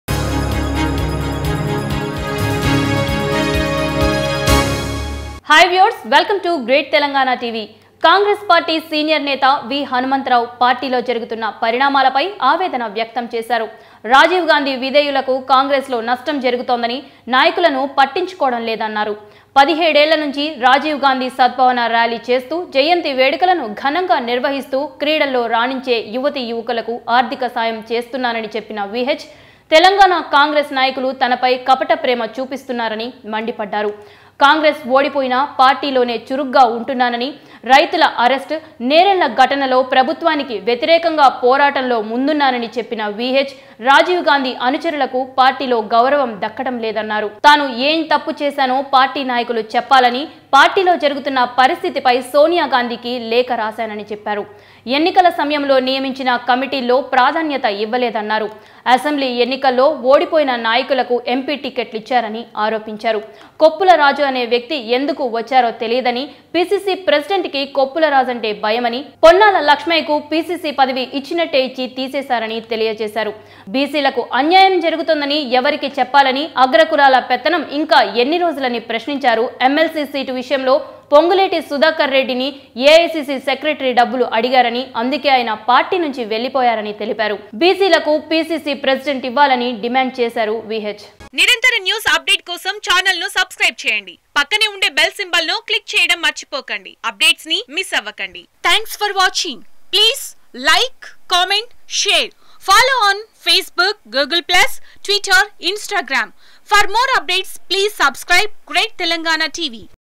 కాంగ్రెస్ పార్టీకి తీరని నష్టం జరుగుతోంది | V Hanmanth Rao | Telangana Congress | GT TV
V Hanmanth Rao | Telangana Congress | GT TV